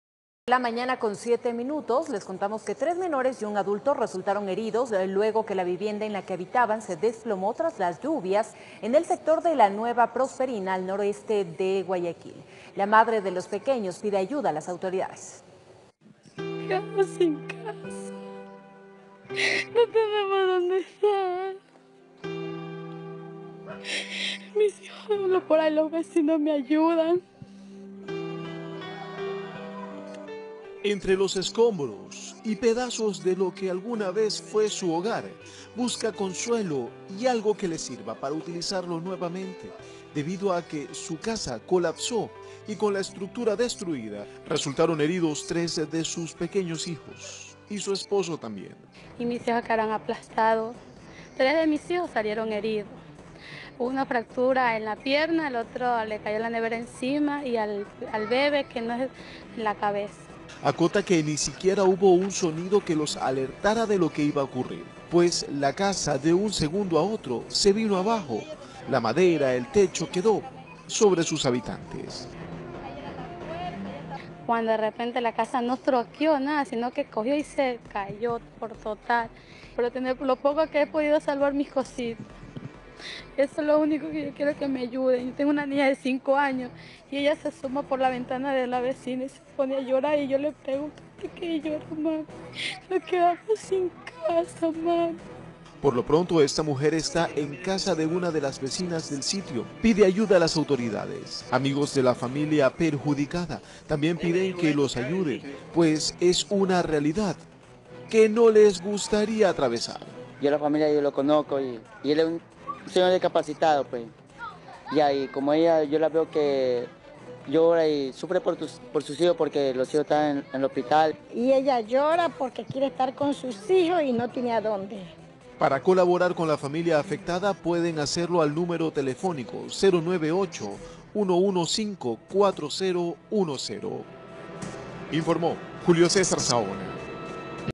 Tres menores heridos luego de colapso de una vivienda en Guayaquil
Tres menores y un adulto resultaron heridos luego que la vivienda en la que habitaban se desplomara a causa de las fuertes lluvias en la ciudad de Guayaquil. La madre de los pequeños pide ayuda a las autoridades